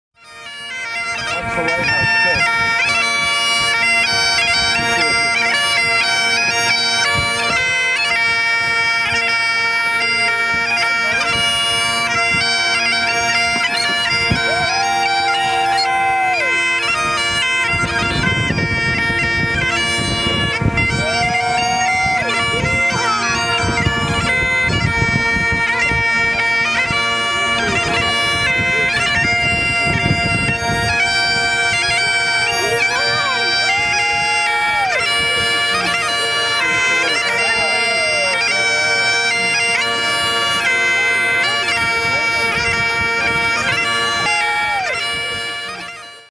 Stephen Taber Crosses Finish Line.mov
As many of the readers of my novel know, schooners play a big part in the setting of The Ghost Trap and I've been lucky to know more than a few captains of windjammers in midcoast Maine. I was invited to hop aboard The Stephen Taber for a first-ever schooner showdown between The Taber and The Lewis R. French, two of America's oldest working wooden schooners, both which happen to be 140 years old. Here is the footage of The Taber crossing the finish line first; Capt. Ken Barnes is playing the bagpipes.